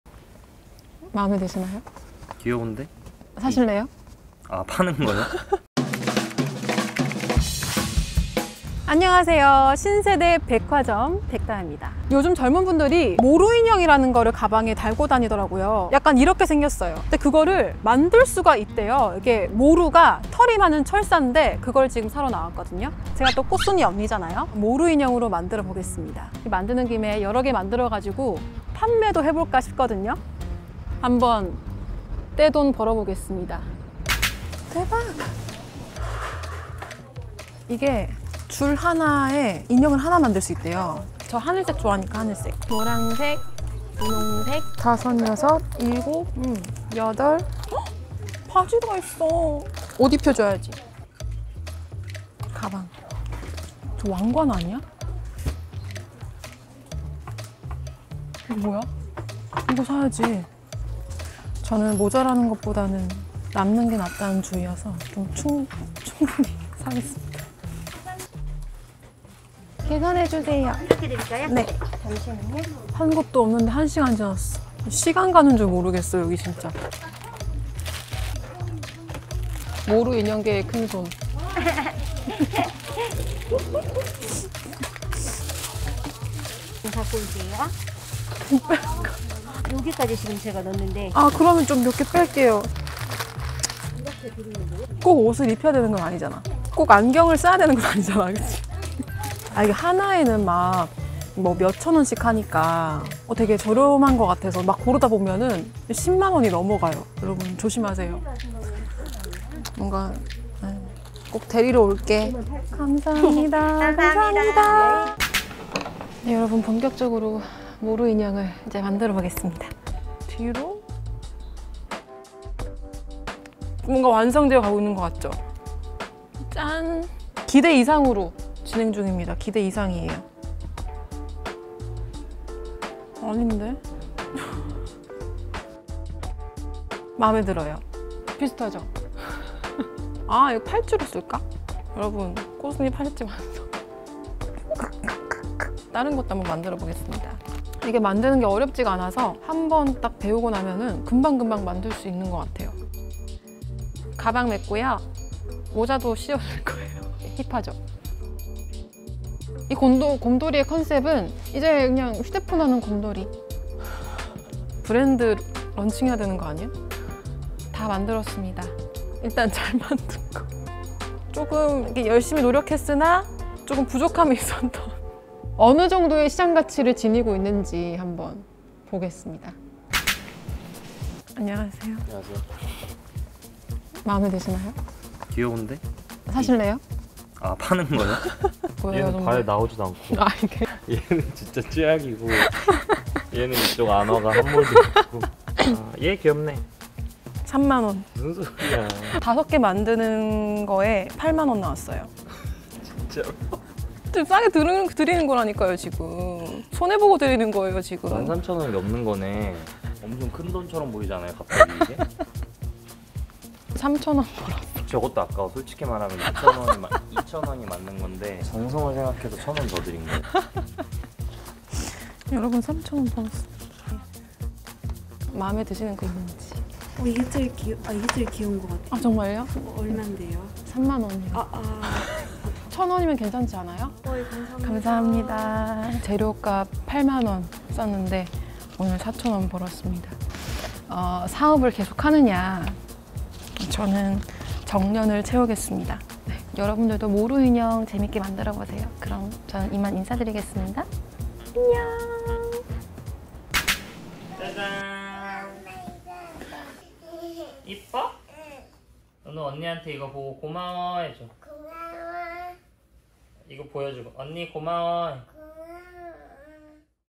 이게 돈이 될까?ㅣ신세대 백화점 EP.21
요즘 지나가면 하나씩 보이는
초귀염둥이 모루인형🧸

직접 만들어볼 수 있다고 해서
동대문시장 가서 재료도 사고🛍️

꽃순이(다혜 아나운서 반려견🐶) 닮은
모루인형 만들기에 도전해 봤습니다!

신세대 백화점은 여러분들의 좋아요(❤️)로 힘을 받습니다!